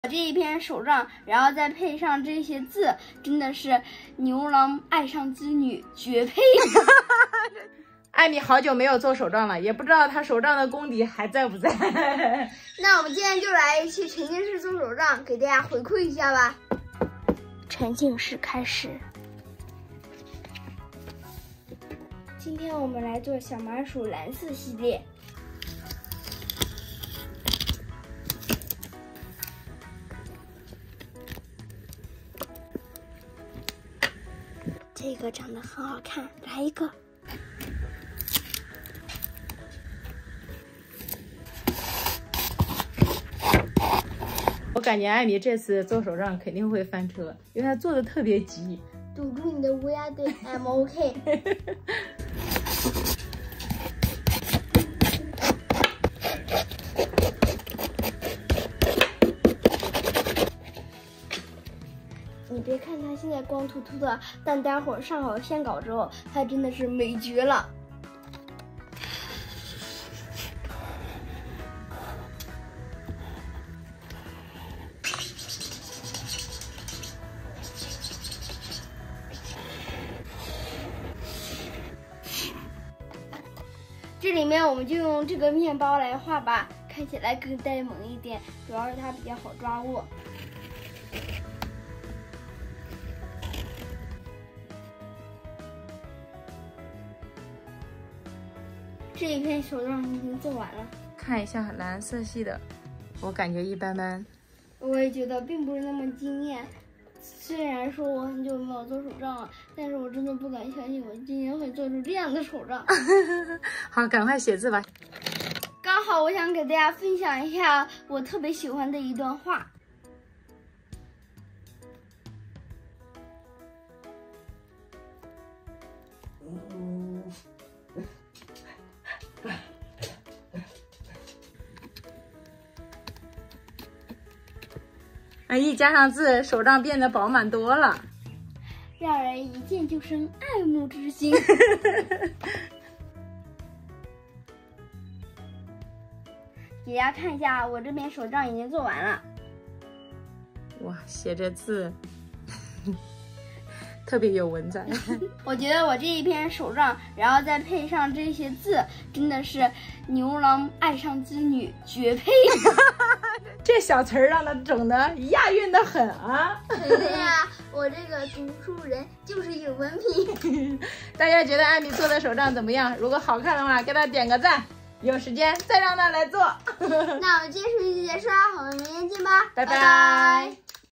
艾米时隔二月终于做手帐了，妈妈嘲笑技术下滑，但文字内容翻盘了
这里是新的开始，和我一起成长吧